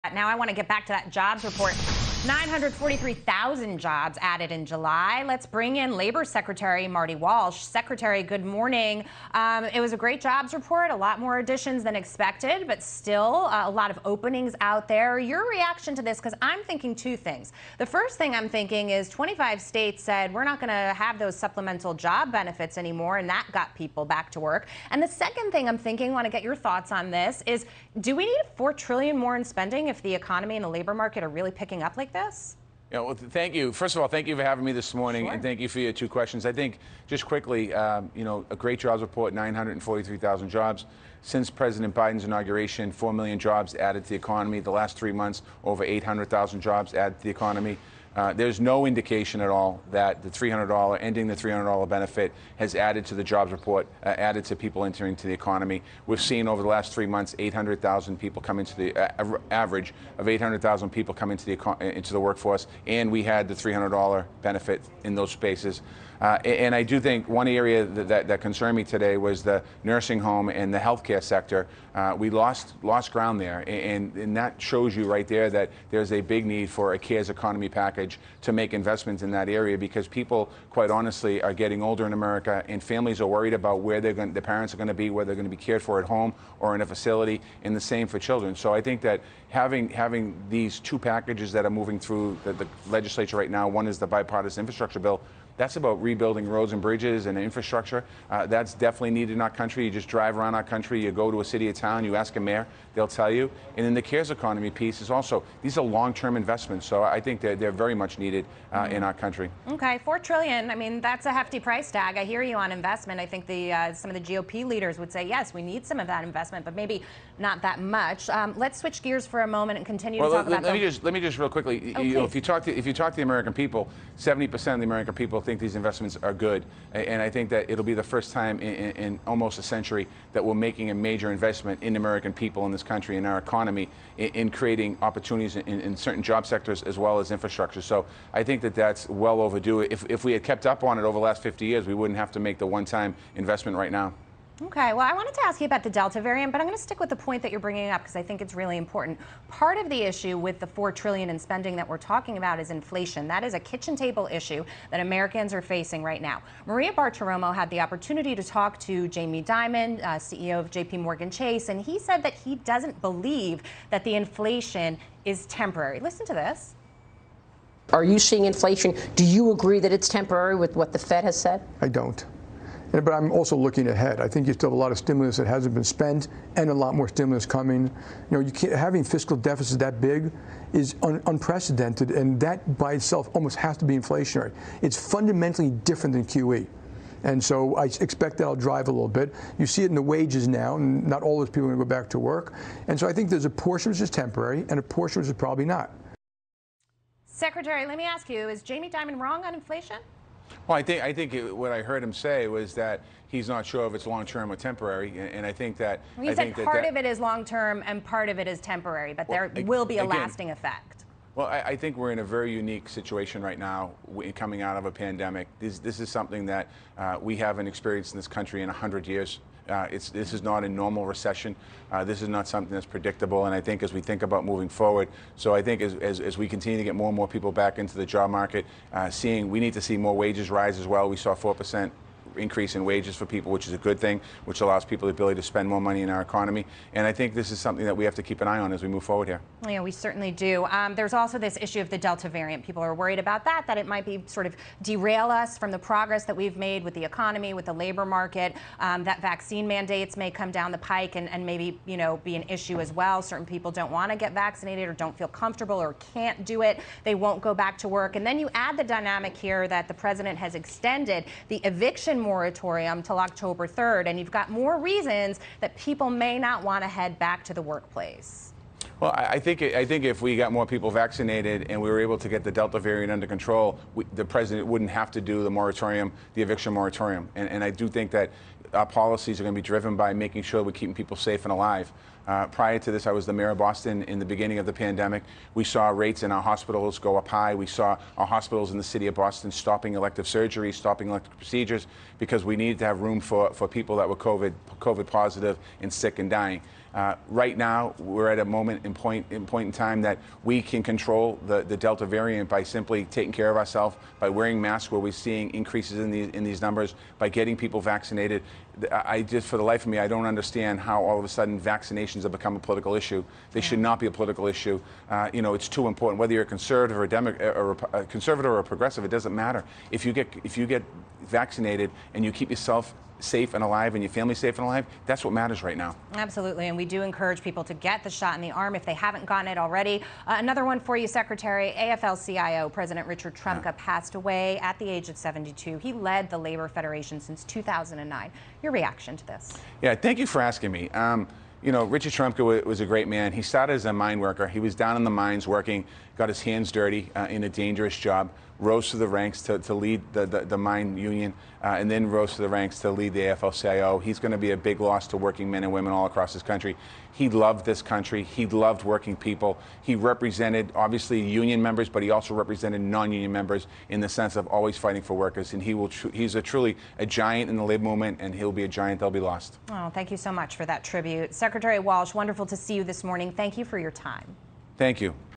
Labor sec on 'great' July jobs report: No sign cutting $300 benefit boosted numbers
Labor Secretary Marty Walsh argues 'there is no indication at all' that ending the boosted unemployment benefits 'has added to the jobs report.'